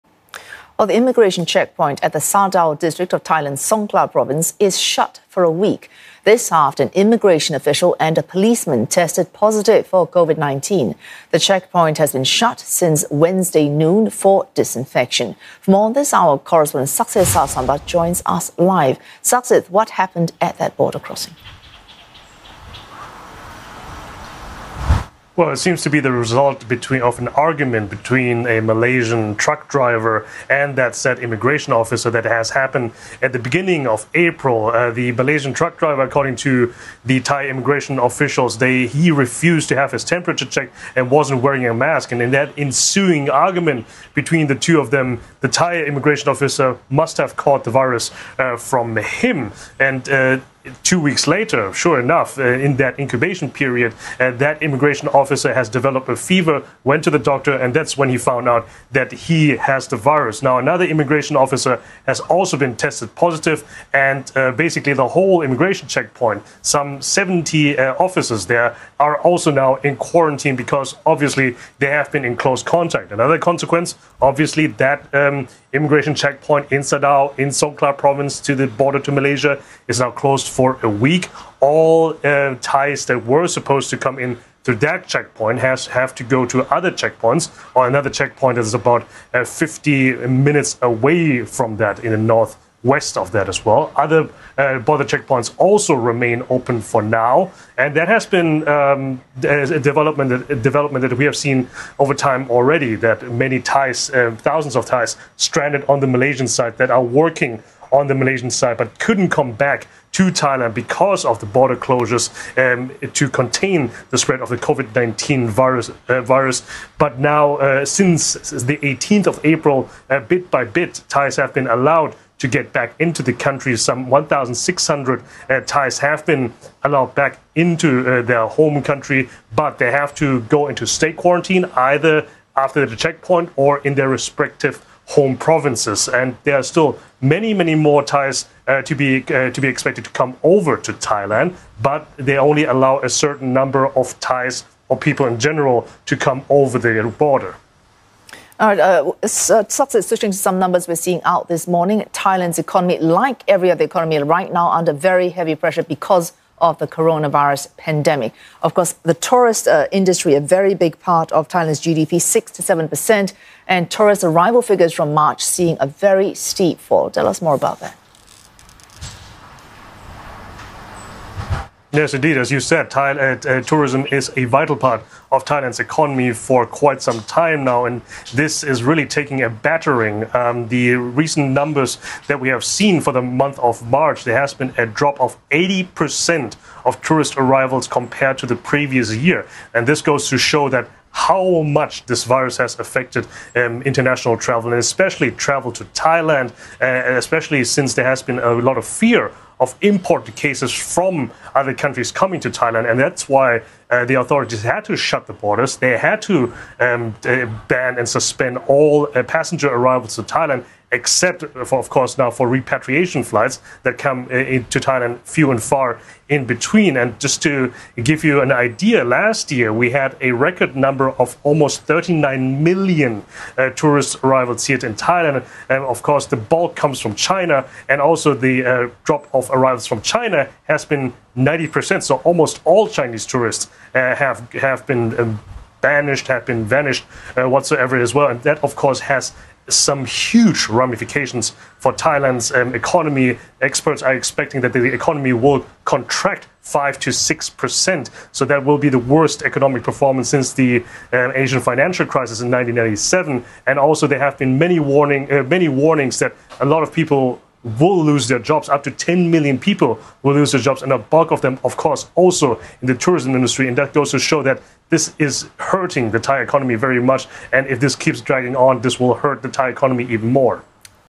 Border checkpoint between Thailand, Malaysia shuts after officer tests positive for COVID-19
The immigration checkpoint at the Sadao district of Thailand’s Songkhla province has been shut for a week after two officers tested positive for COVID-19. CNA's Saksith Saiyasombut has the details on the impact of the closure.